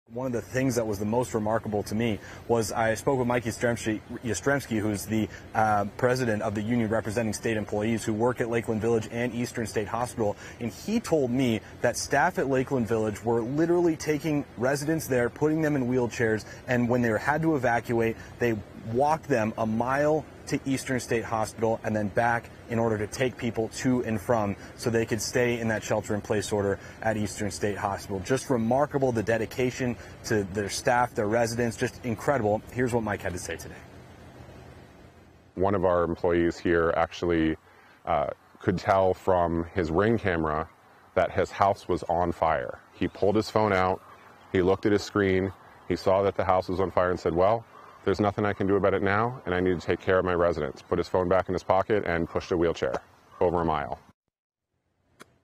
This is why we do what we do.
When the GrayFire threatened residents of Lakeland Village, WFSE/AFSCME members didn't think twice. It's acts of heroism like this, some seen some unseen, that define the work our members do every day.